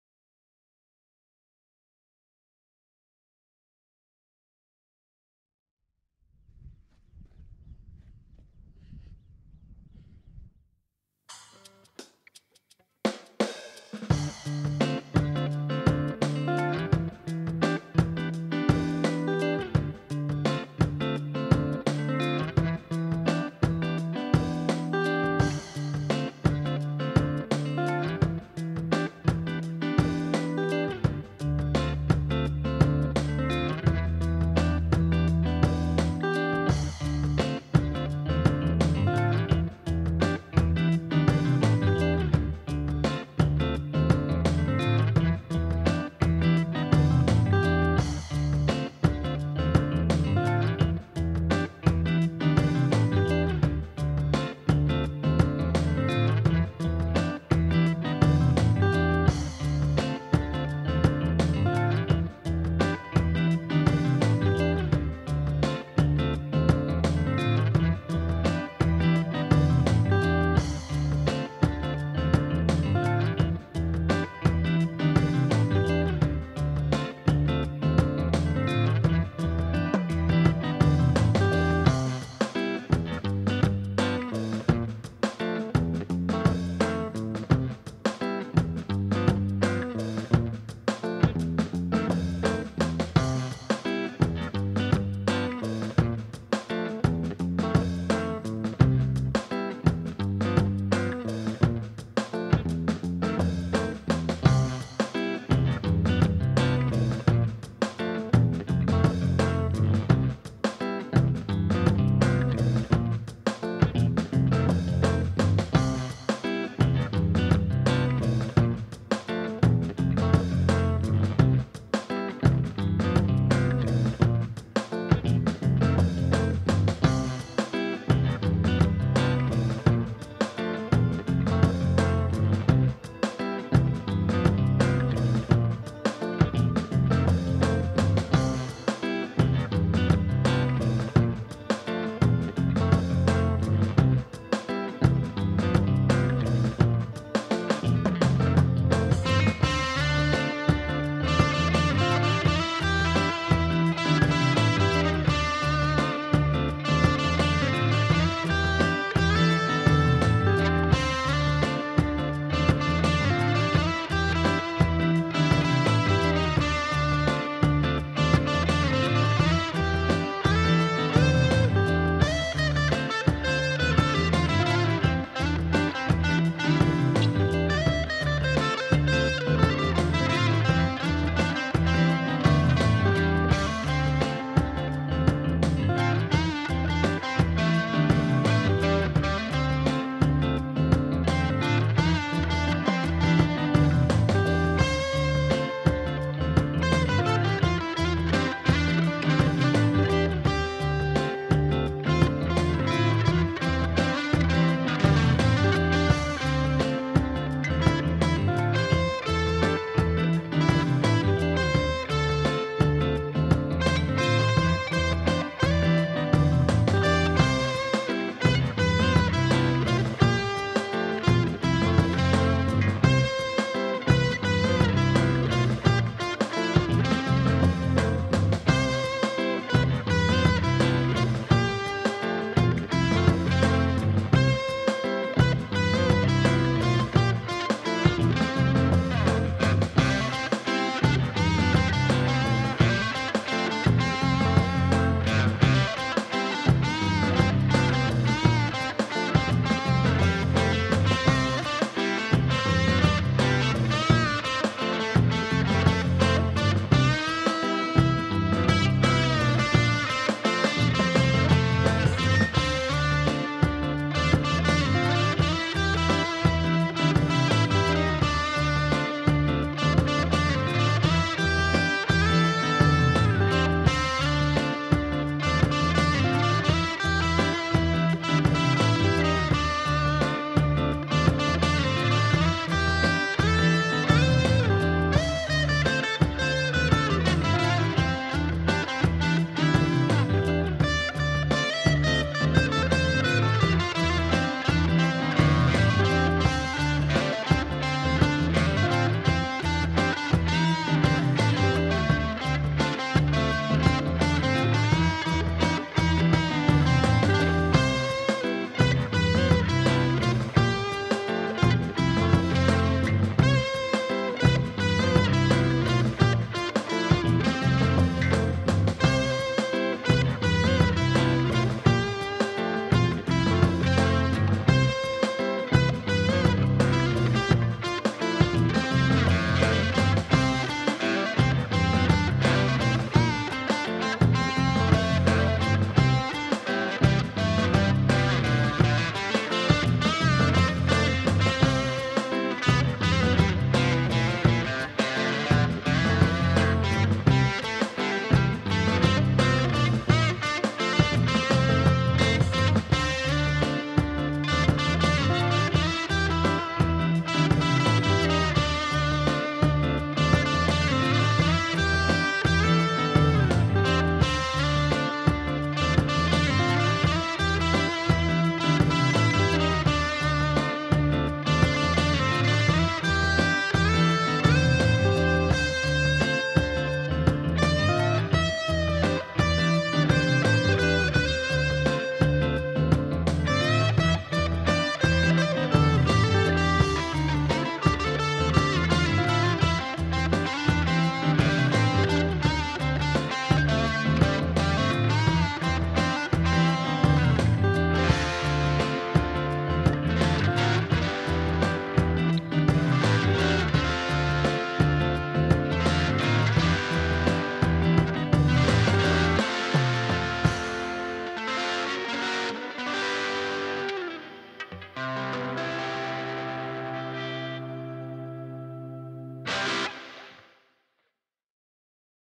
Sunday Breakfast Jam no. 16
A funky tune performed on my private beach ;)

Life's a beach sometimes... Have a great Sunday everyone!